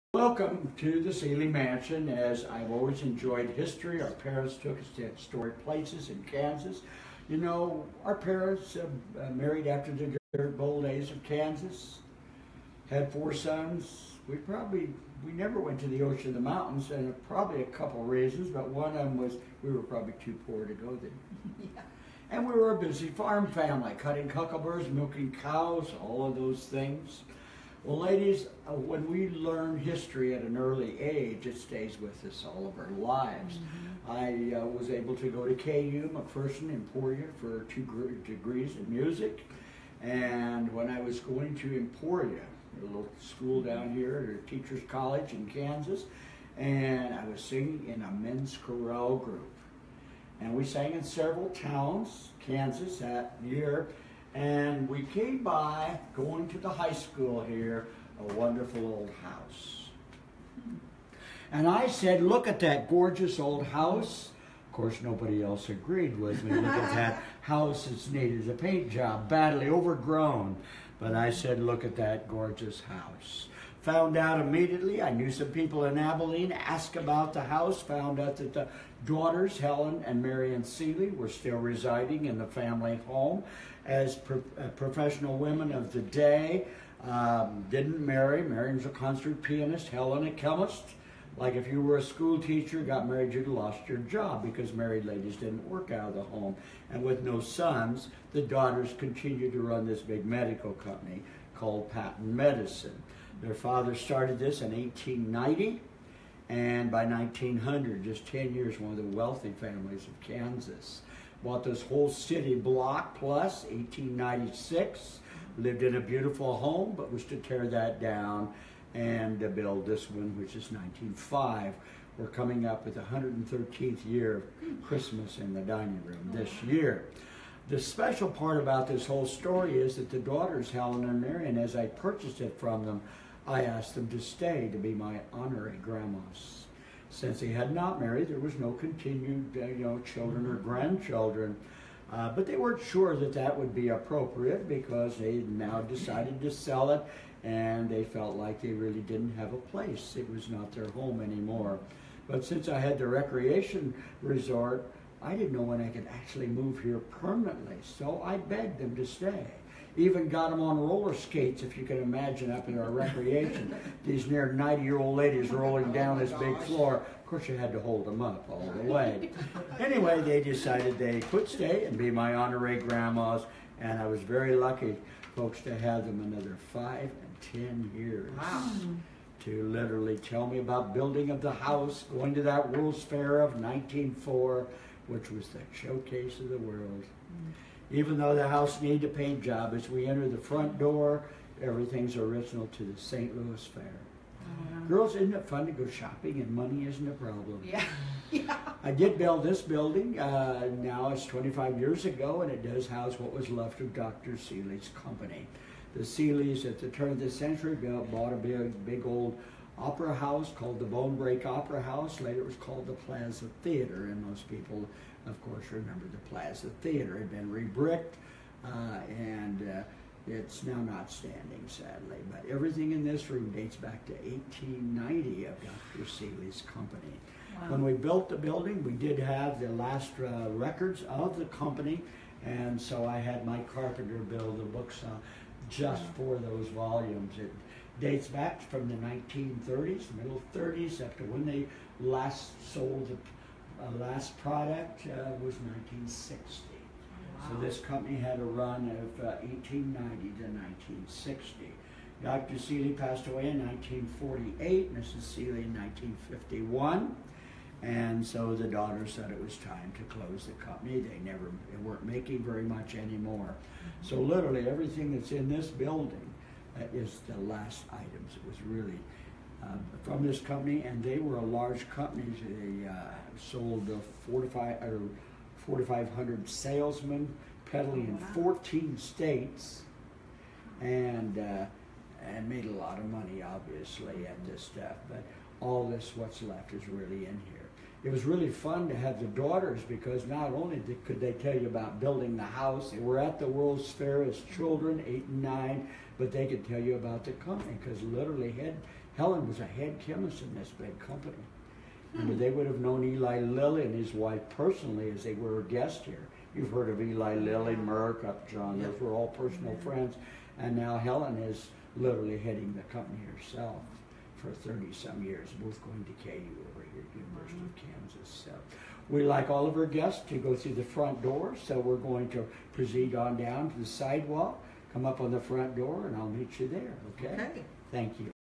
Abilene Travel Talks: Seelye Mansion History
Ever wonder about the story being Abilene's historic Seelye Mansion? Hear the history live with owner Terry Tietjens

Follow Visit Abilene Kansas for more great videos!